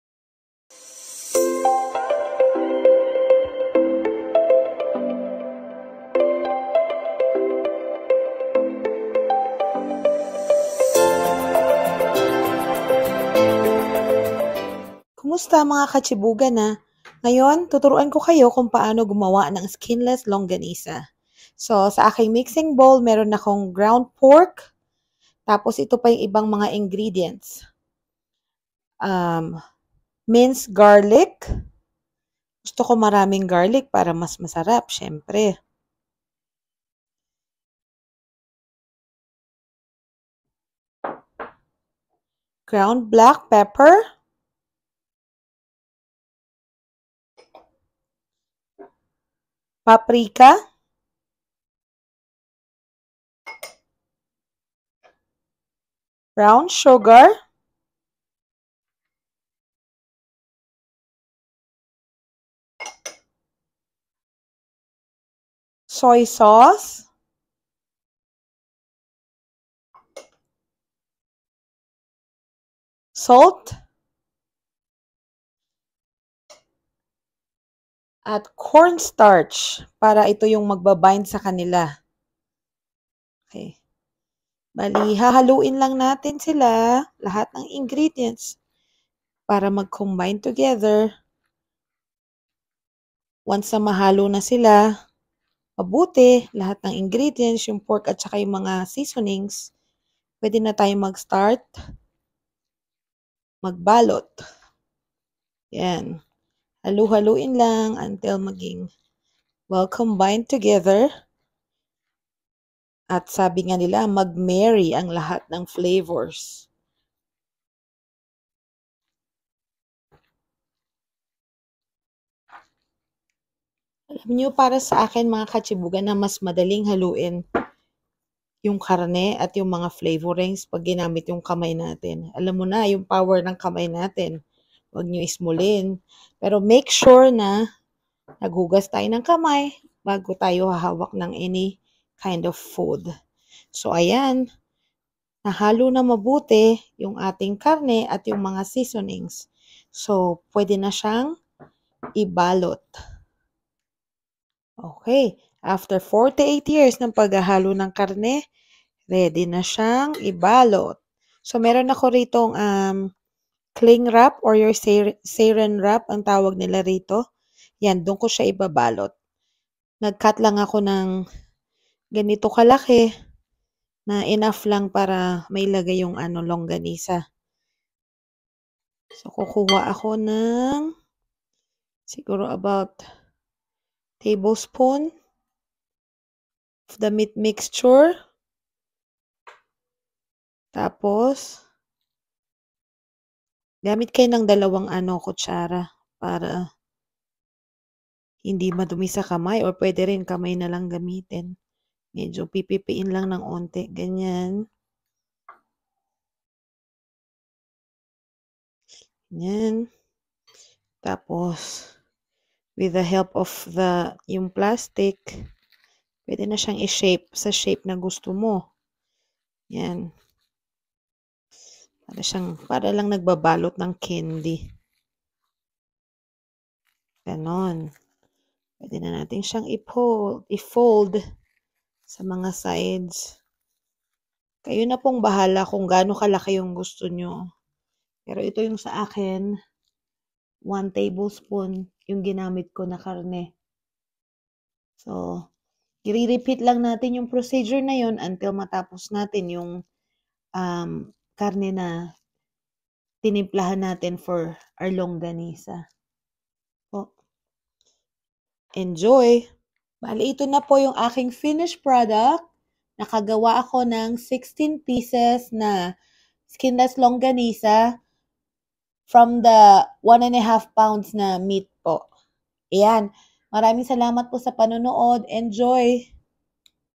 Skinless Longanisa (Filipino sausage)
Skinless Longanisa is a type of
Filipino sausage that doesn’t have a casing, it is usually individually wrapped in a plastic or parchment paper.  This Filipino sausage can either be garlicky or sweet.

Ingredients:

1 1/2 lbs ground pork
1 tsp ground black pepper
1/2 cup brown sugar
3 tbsp minced garlic
2 tbsp soysauce
2 tsp salt
2 tsp paprika (for added flavor and color)
2 tbsp cornstarch

Direction:

1.  In a large mixing bowl, place the ground pork.

2. Add in all the seasonings to the ground pork. (ground black pepper, brown sugar, minced garlic, soysauce, salt, paprika, and cornstarch) Mix them all together until all the flavors are combined very well with the ground pork.

3.  Cut small pieces of Saran Wrap/ Cling wrap, enough size that could fit in and wrap your longanisa.

4. Scoop a tablespoon of the meat mixture, place it at the middle of the plastic wrap.  By using 2 tablespoons shape the meat mixture like a small log, then wrap them individually as if you are wrapping a candy.

5.). Keep the individually wrapped longanisa on the fridge or freezer for longer shelf life. Enjoy!

Note:  Out of the 1 1/2 lbs of ground pork I was able to make 16 pieces of longanisa.